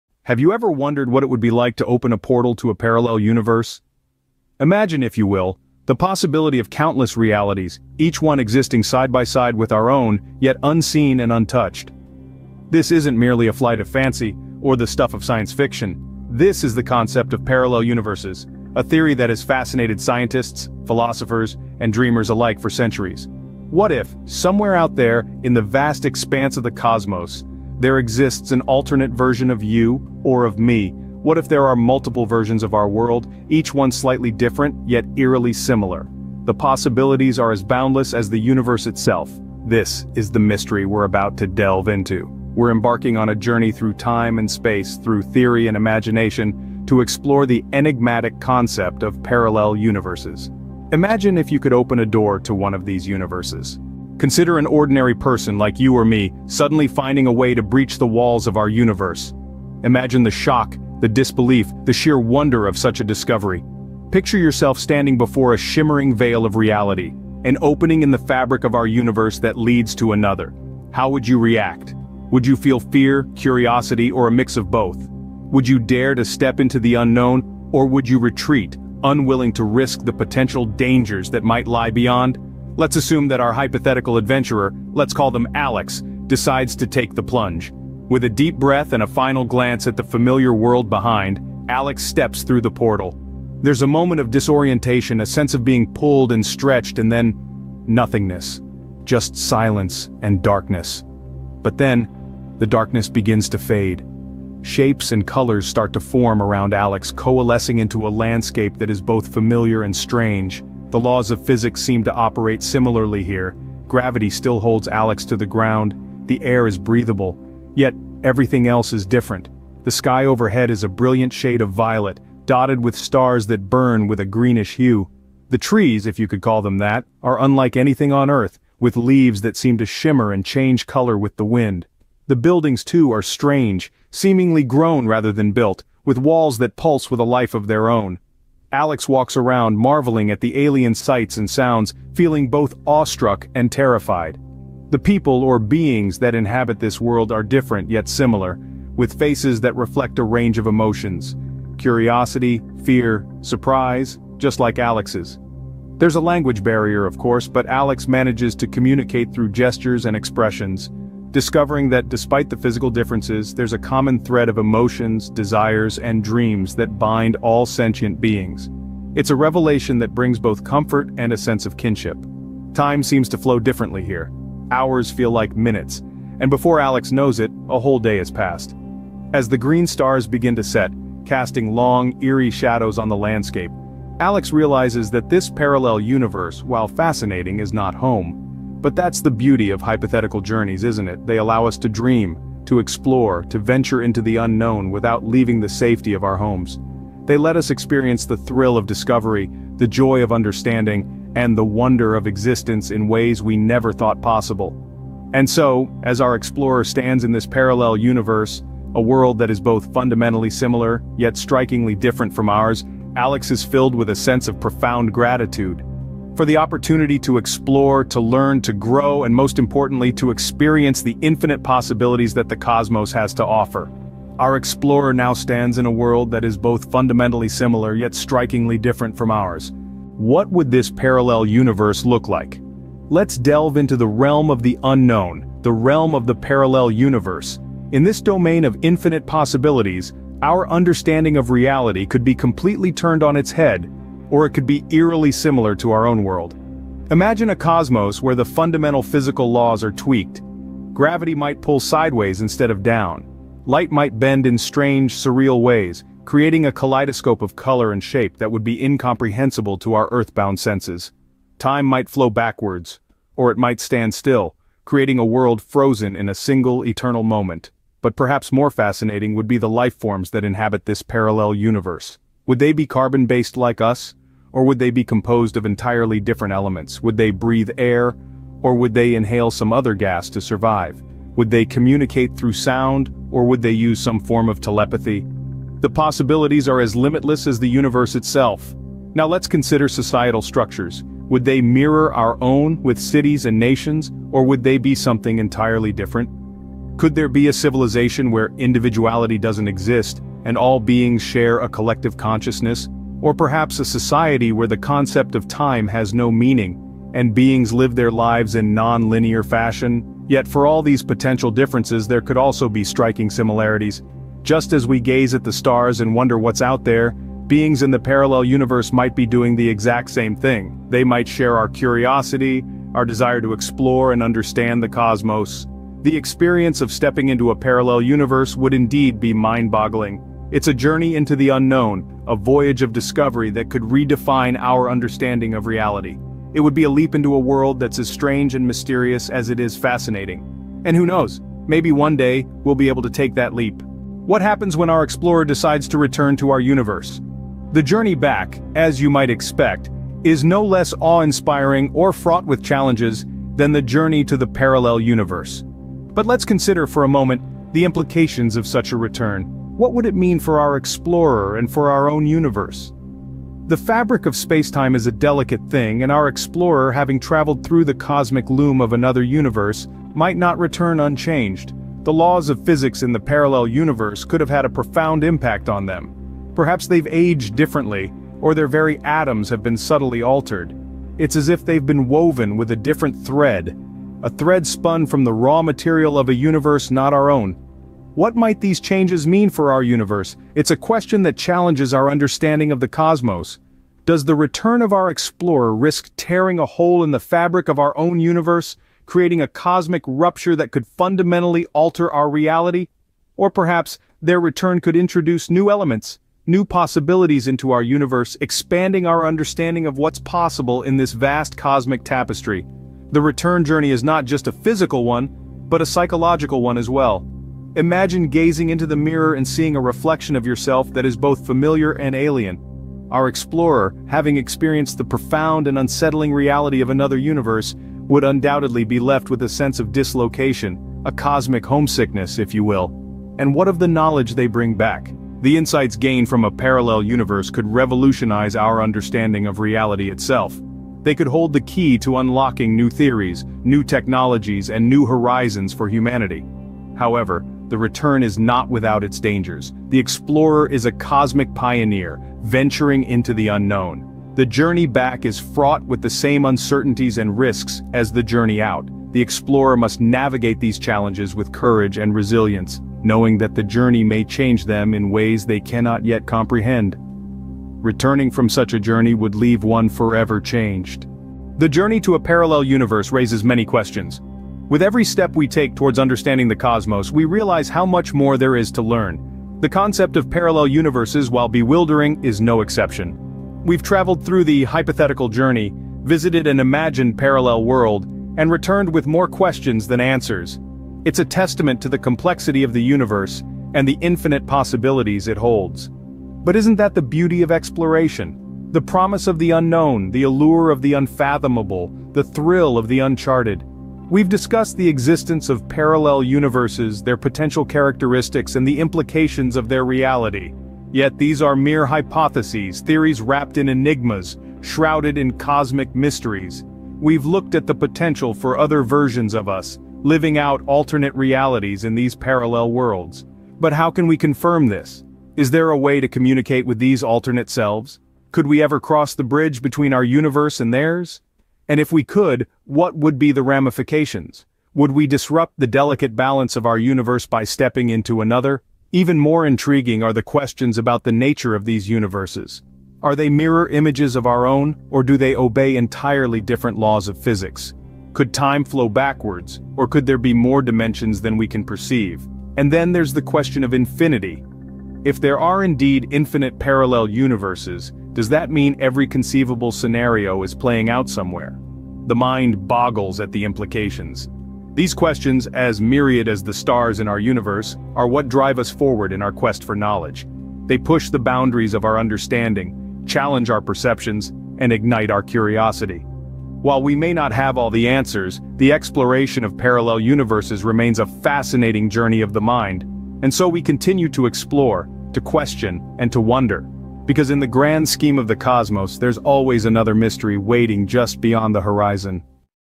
What if We Could Unlock the Gateways to Parallel Universes?
Embark on a mind-bending journey as we delve into the fascinating concept of opening the portals to parallel universes! 🌌 Join us in this thrilling exploration of the hypothetical, where we unravel the mysteries of alternate realities and ponder the unimaginable possibilities that lie beyond our own. Prepare to expand your perspective and challenge the boundaries of conventional thinking as we contemplate the question: What if we could unlock the gateways to parallel universes? Buckle up for a cosmic adventure that will leave you questioning the very fabric of reality itself. 🚀